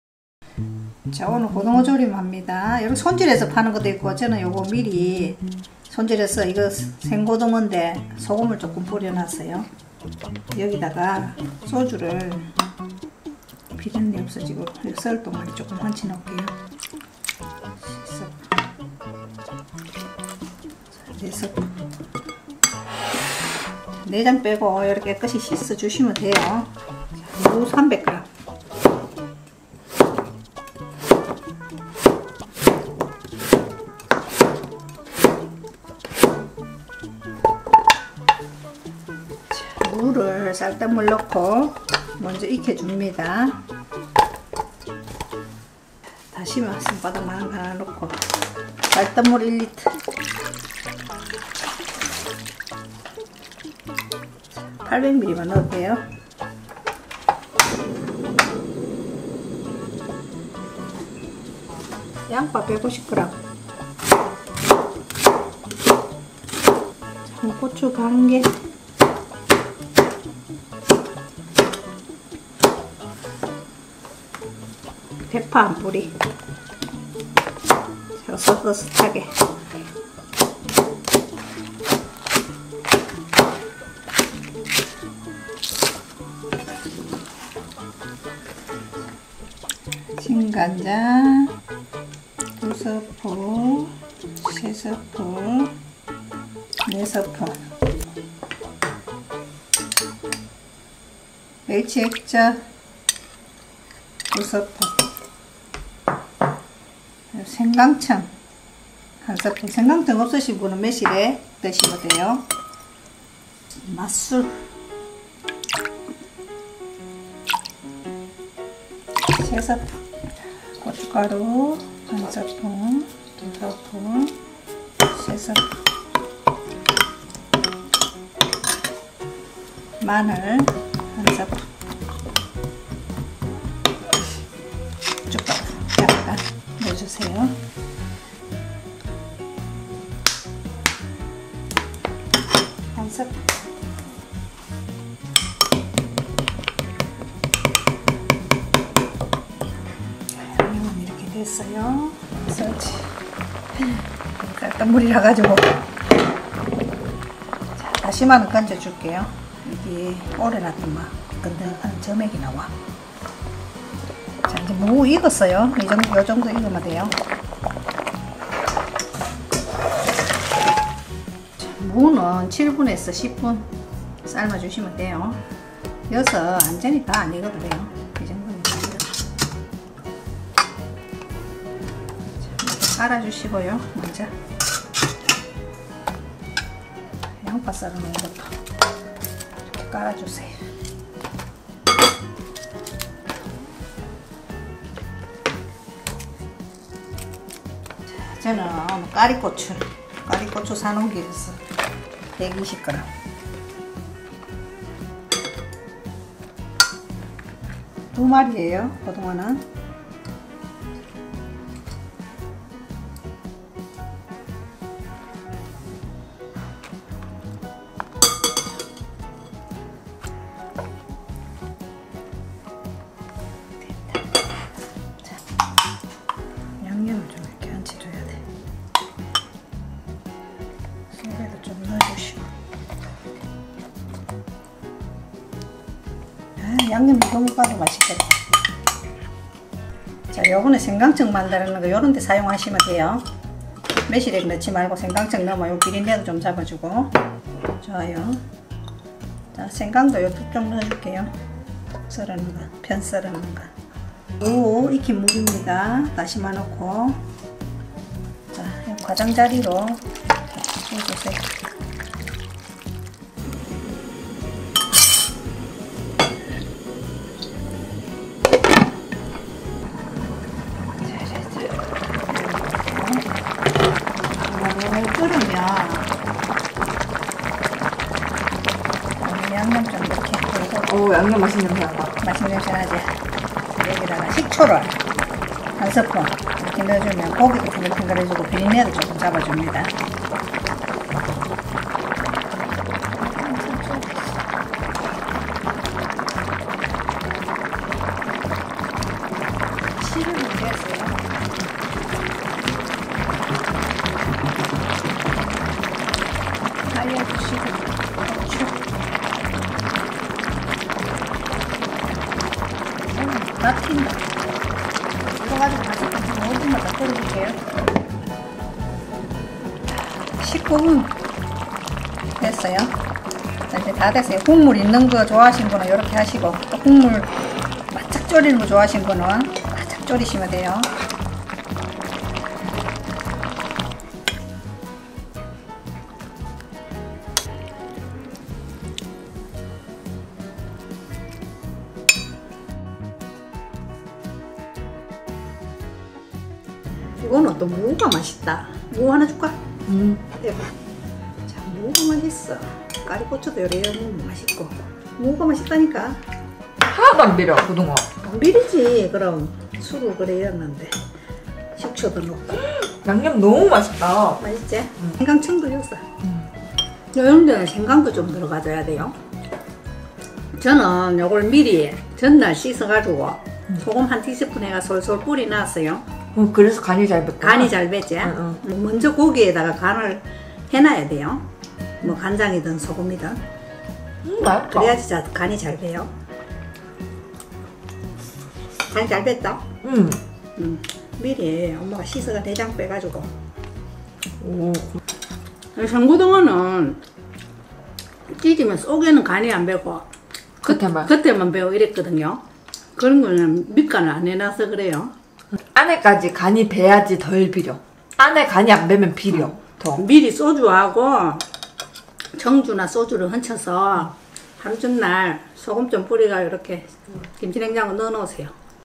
통조림 고등어조림 먹지마세요! 생물 고등어조림이 훨씬 맛있거든요.
-재료-
고등어 2마리, 소주 4스푼, 무 300g
양파 150g, 쌀뜨물 800ml, 다시마 1장
홍고추 반개, 진간장 4스푼, 식초 1스푼
멸치액젓 2스푼, 생강청 1스푼, 생강 2쪽
맛술 3스푼, 고춧가루 3스푼, 꽈리고추 120g
다진마늘 1스푼, 후춧가루 약간, 물엿 1스푼

-레시피-
1. 생고등어를 하루 전날 손질해서 씻어서 소주, 소금으로 밑간을 해두세요.
2. 고등어에 소주 4스푼 뿌려주세요.
3. 무, 양파, 대파, 고추를 썰어주세요.
4. 냄비에 무를 먼저 깔고 쌀뜨물 800ml, 다시마 1장 넣고 10분만 먼저 끓여주세요.
5. 진간장 4스푼, 멸치액젓 2스푼, 생강청 1스푼, 맛술 3스푼, 고춧가루 3스푼, 다진마늘 1스푼, 후춧가루 약간, 물엿 1스푼 넣고 양념을 만들어주세요.
6. 무 끓인 육수를 다시마만 건져주세요.
7. 무 위에 양파, 꽈리고추, 고등어를 올려주세요.
8. 양념을 고등어에 올려주세요.
9. 생강 2쪽도 넣어주세요.
10. 팔팔 끓으면 식초 1스푼 넣어주세요.
11. 12분 되면 대파, 고추를 넣어주세요.
12. 총 20분정도 끓으면 완성.